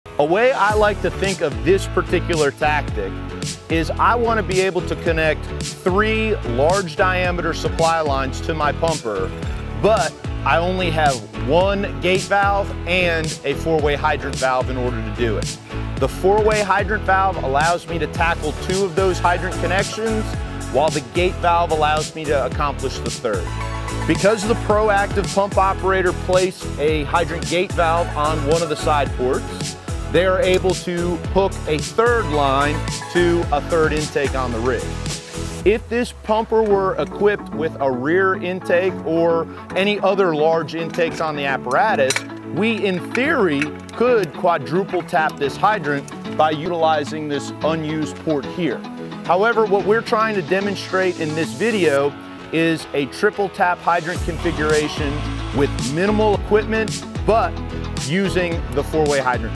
🚒 Maximizing Fire Hydrant Efficiency: Triple Tap Step 4 🚒
🚒 Maximizing Fire Hydrant Efficiency: Triple Tap Hydrant Configuration (Step 4)! 🚒

🎥 In this video, we break down a powerful tactic to connect three large diameter supply lines to your pumper using minimal equipment! 🧑‍🚒 Here's how it's done:

👉 Step 4: Establish third supply line
- 4-Way Hydrant Valve: Tackles two hydrant connections.
- Gate Valve: Manages the third line to the pumper.

FULL EPISODE + CHECKLIST: Hydrant Water Supply homepage

💡 Pro Tip: A proactive pump operator can hook a third line using a hydrant gate valve on one of the side ports. With additional intakes, quadruple tapping is even possible! 💪

🔥 The Goal: Demonstrating a triple tap hydrant configuration with minimal gear using a 4-way hydrant valve for superior fire flows!